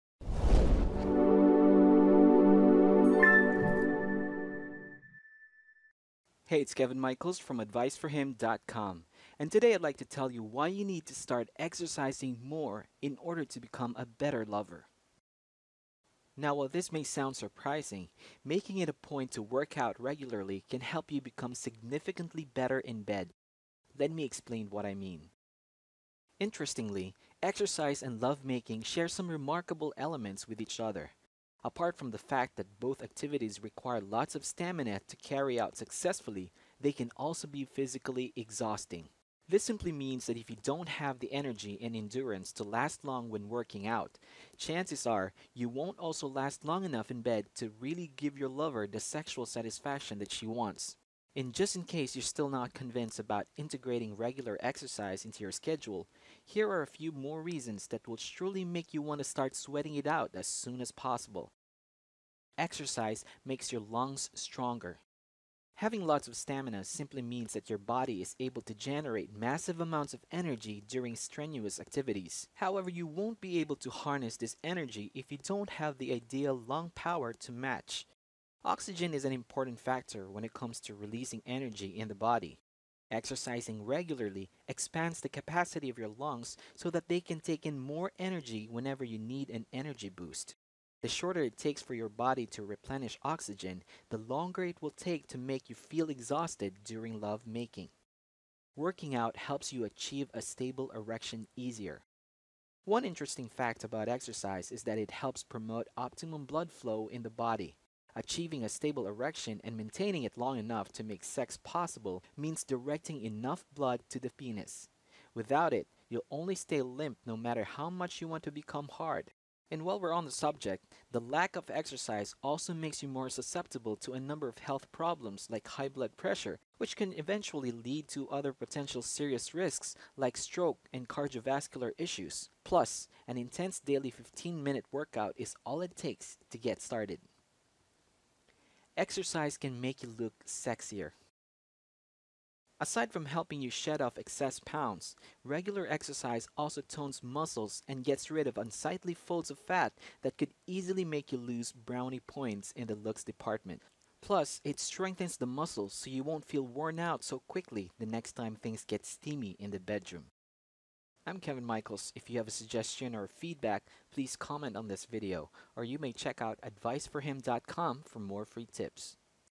Why You Need To Start Exercising More In Order To Become A Better Lover
- Now while this may sound surprising, making it a point to work out regularly can help you become significantly better in bed.